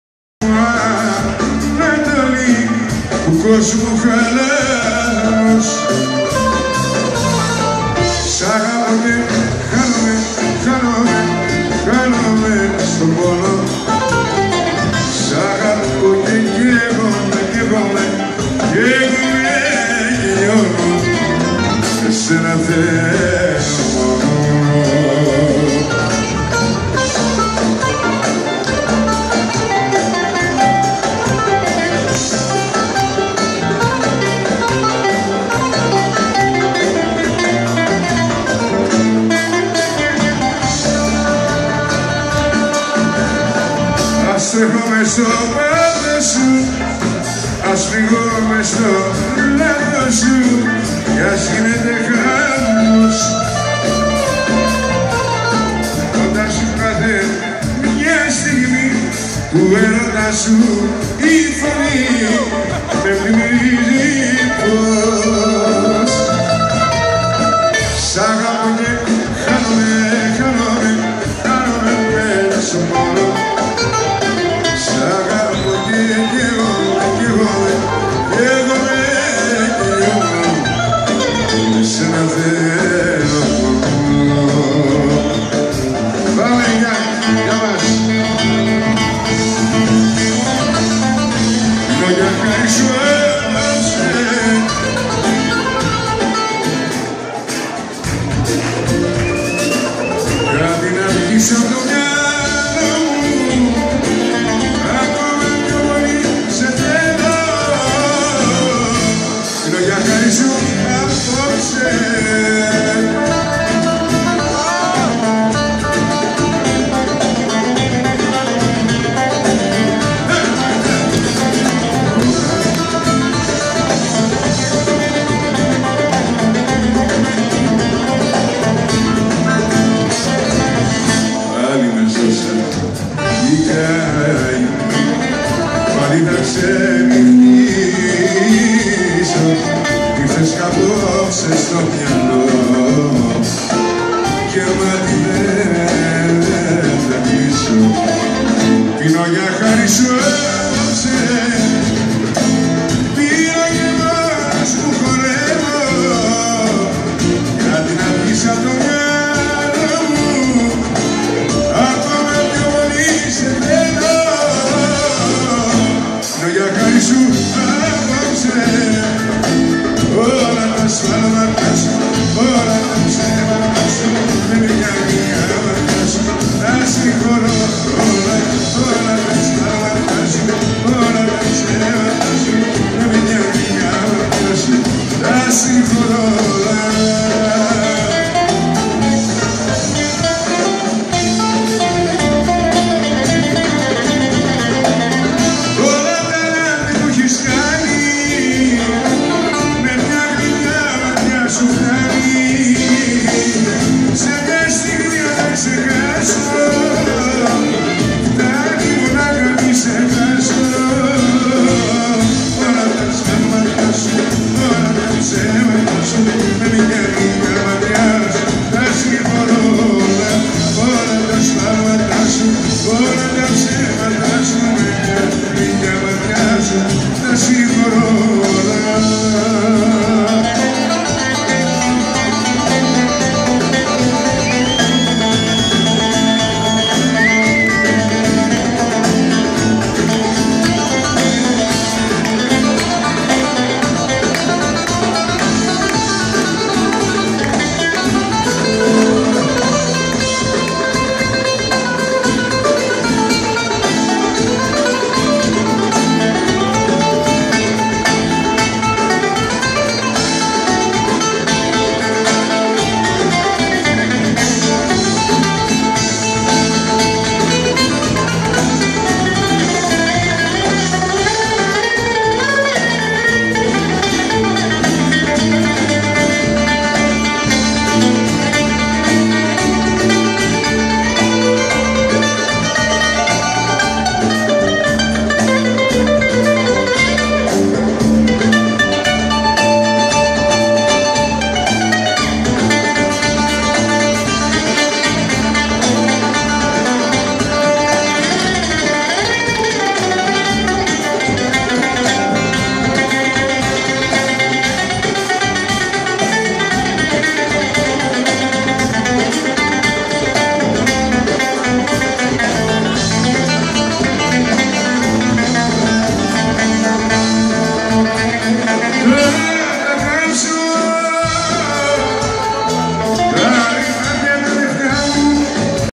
Themis adamantidis in israel in mendis...live part 2
הופעה חיה של Themis adamantidis במנדיס בר יהוד. ערב יווני משובח עם מוזיקה טובה ואוכל טוב ואנשים טובים.